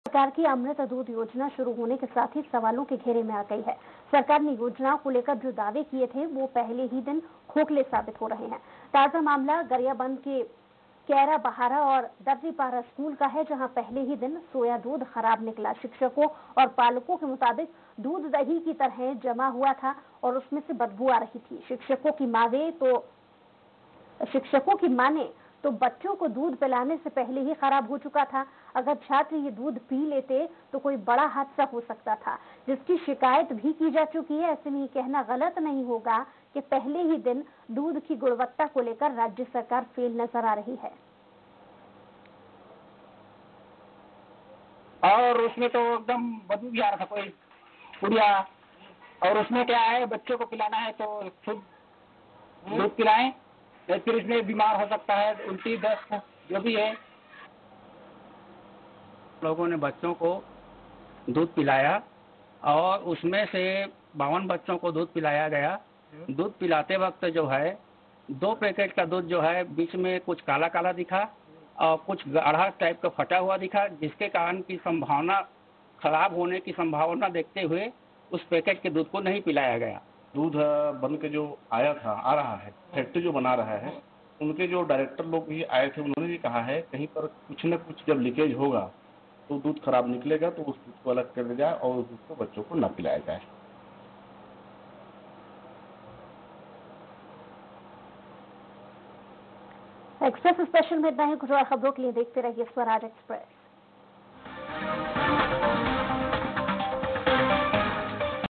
गरियाबंद: मुख्यमंत्री दुग्ध वितरण योजना पर उठे सवाल
Gariaband: Questions raised on Chief Minister's milk distribution scheme